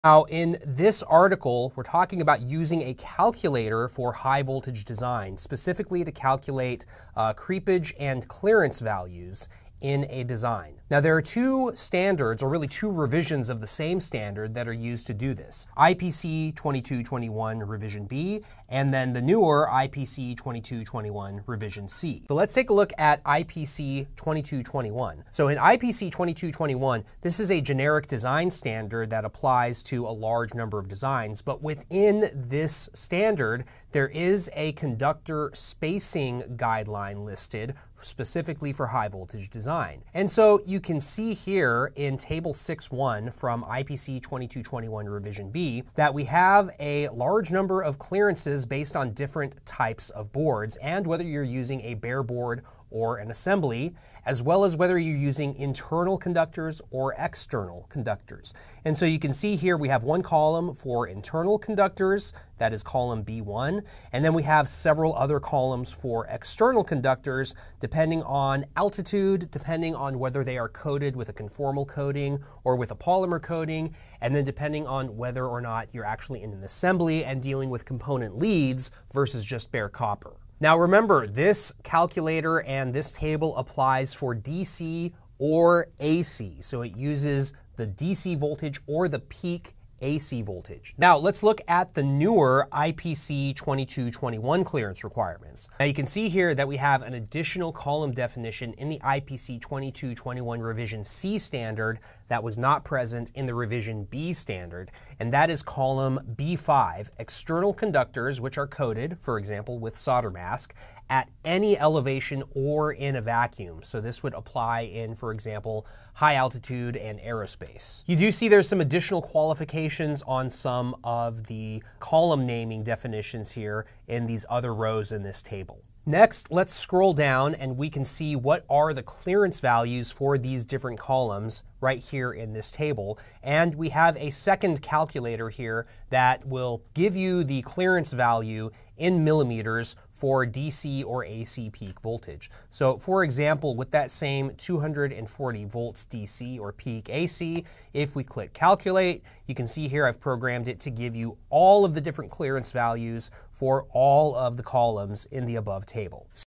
IPC 2221: Clearance Standards for High Voltage PCB Design
Dive deep into the critical world of high voltage PCB design with our comprehensive guide to IPC 2221 clearance standards. Learn how to accurately calculate creepage and clearance values for different board types, conductor configurations, and voltage levels using professional design techniques.

Share your thoughts and questions in the comments below!